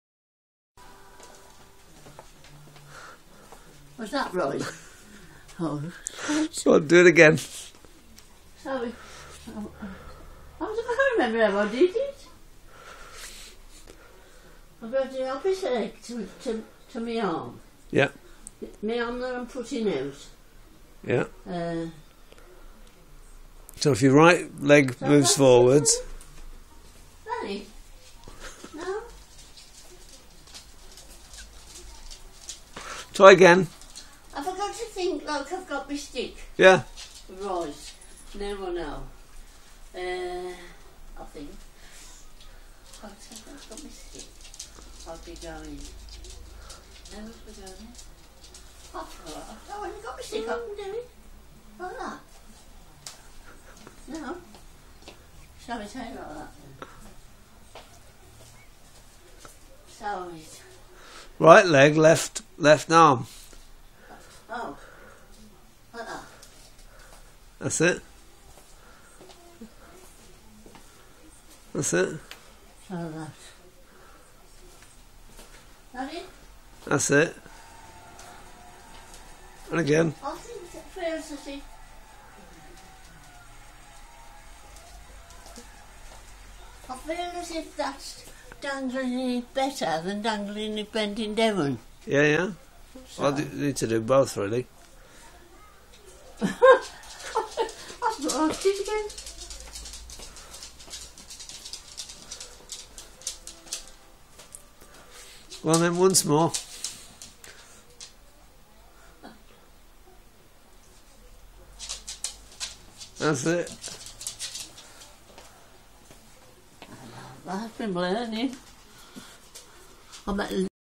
pearl learning to walk
Pearl trying to get a grip with right leg left arm , who would think it would take so much concentration.